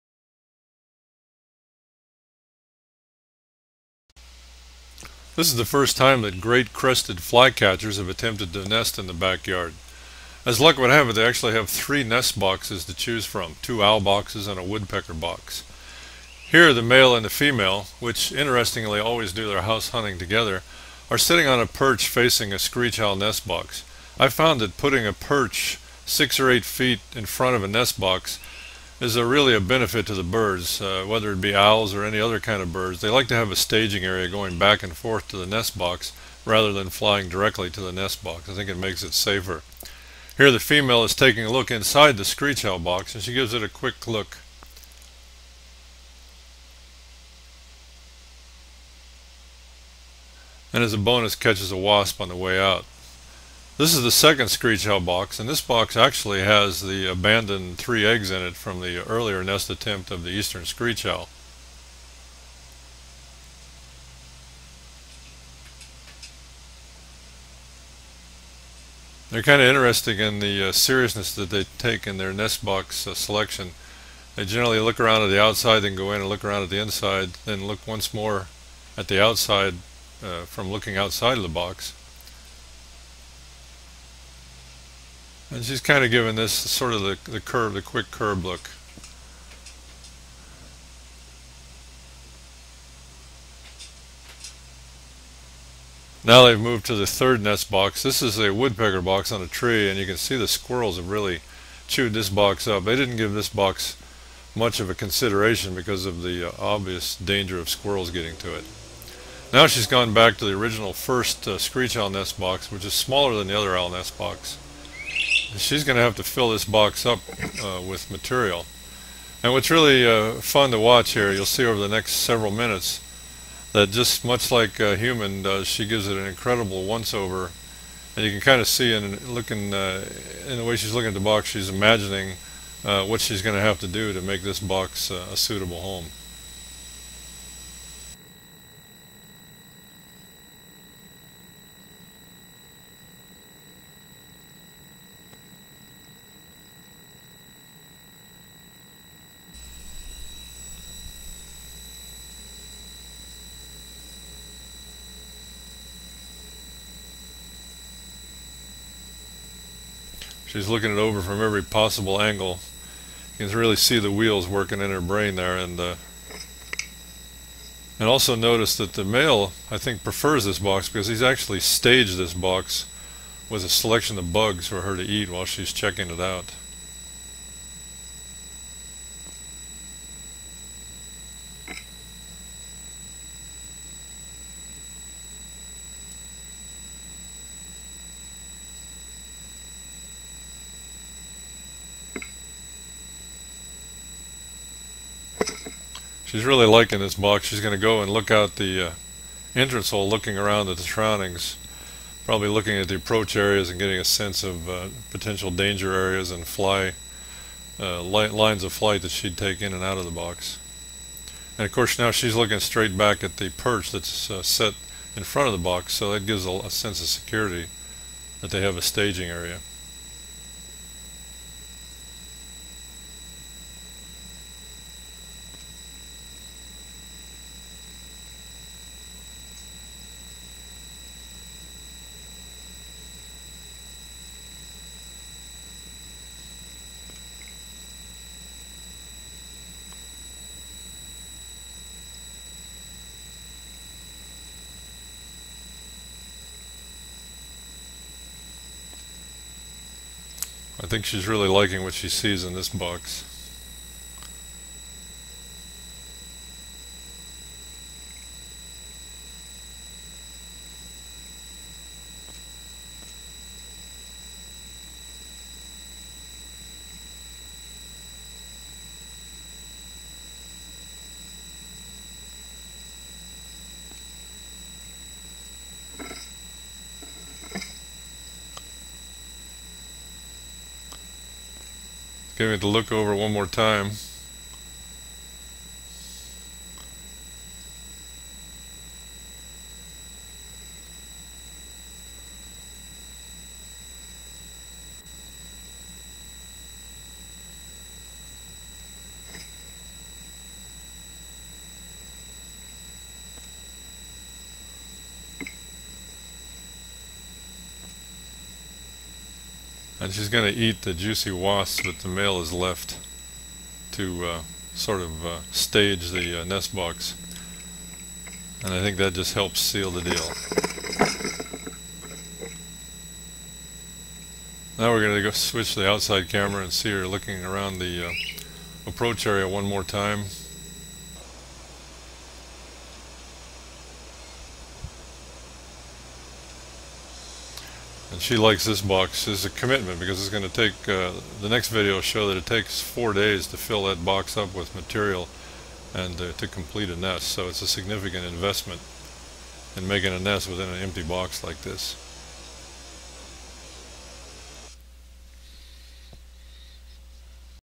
Great Crested Flycatcher Nest Box Documentary
Mini- Documentary about a pair of Great Crested Flycatchers inspecting two owl nest boxes and a woodpecker nest box before deciding where to build their nest and raise their family. Watch as this fascinating Great Crested Flycatcher couple go house hunting in a buyers market - its all about location, location, location! Also note the importance of placing a post for perching in the open near the nest box - birds really like this feature for ease of access, hunting and security.

Note: The nest box camera failed due to a lightning strike, but four of the five eggs hatched and four baby flycatchers successfully fledged during the fourth week of May!